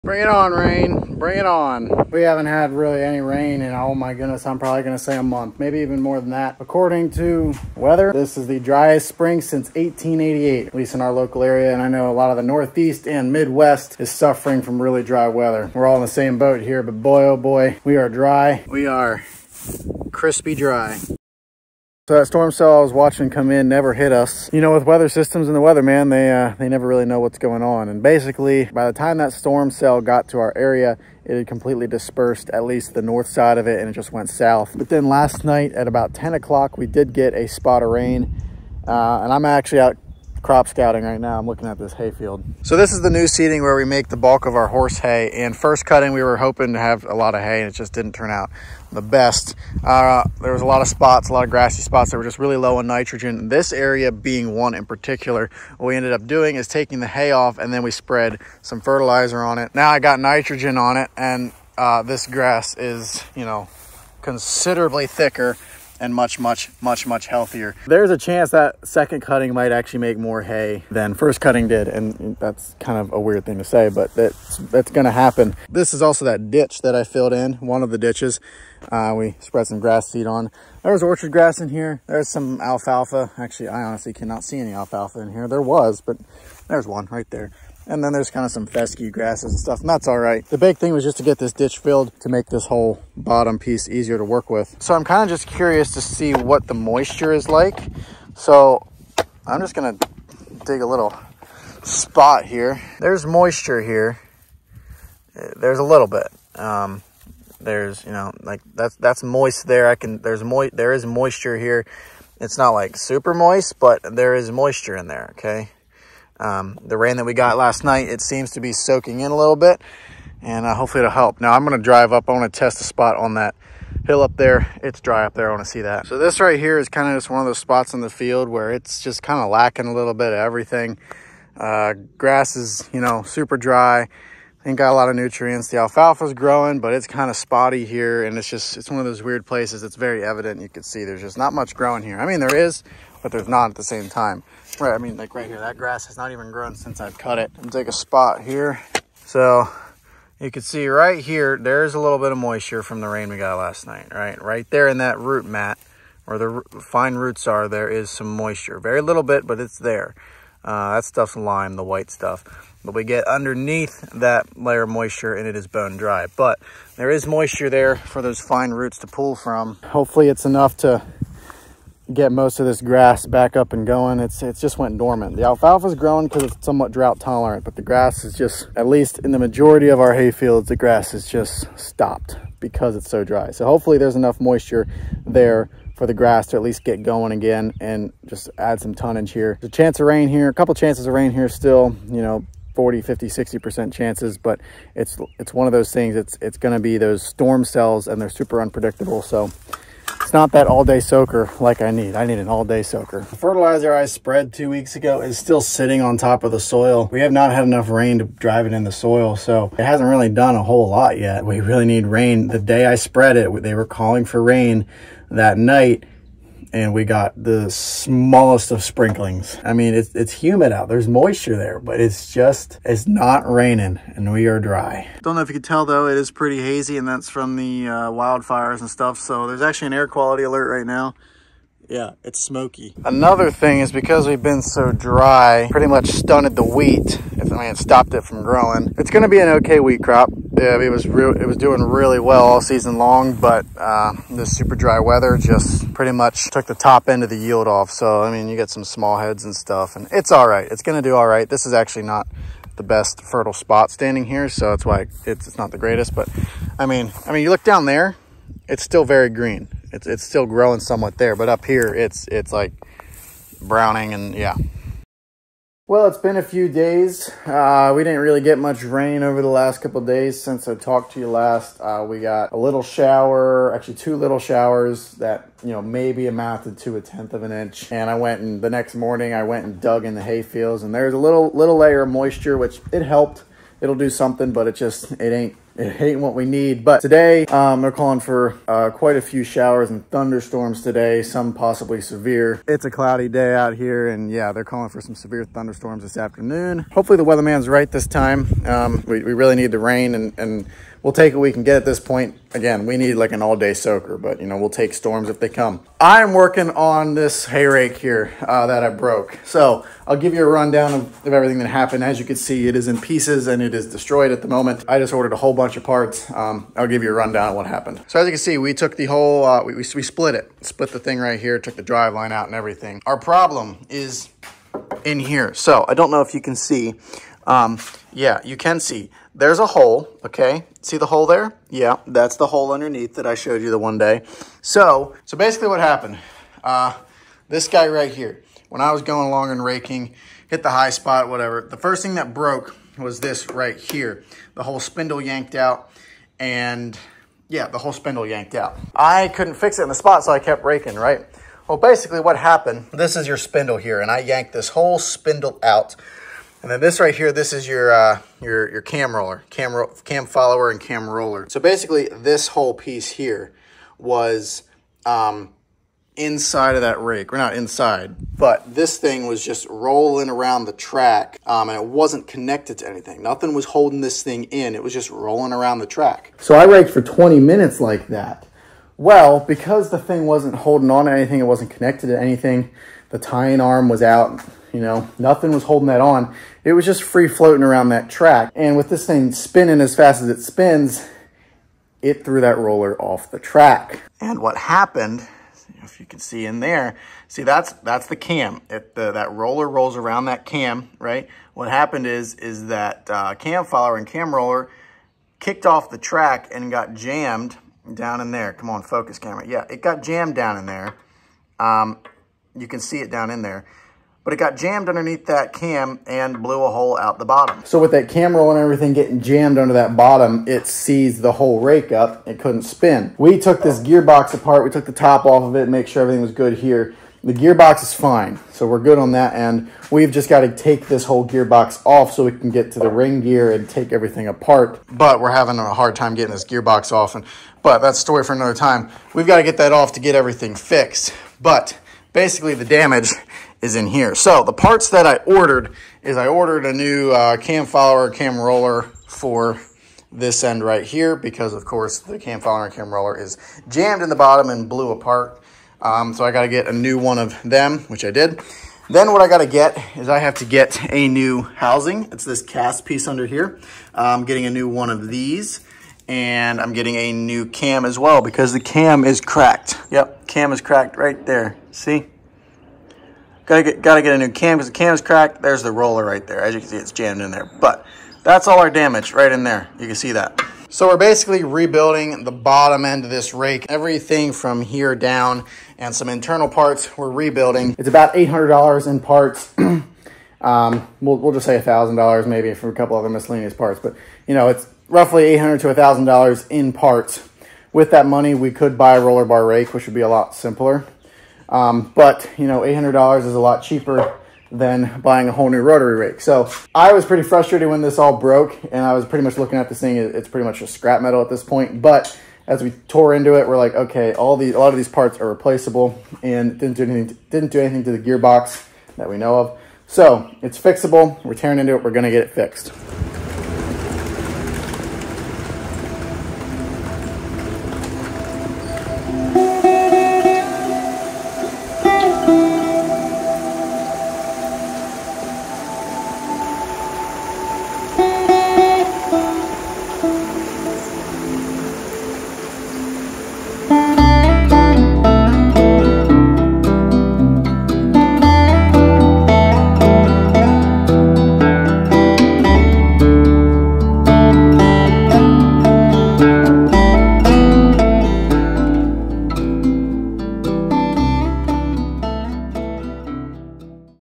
The hay rake is in 1 MILLION PIECES!!!! (2023 DROUGHT)
The 2023 drought has taken a number on the local crops including our hay! Fortunately, we got a good shot of rain and everything is Back on track! In other news...the hay rake is in a million peices!

⬇️ CAMERA GEAR LIST⬇️
CAMERA GEAR:
iPhone 14 PRO
GoPro Hero 7
GoPro Clamp/Extension
Muddy Camera Arm
RODE Mic
iPhone Mic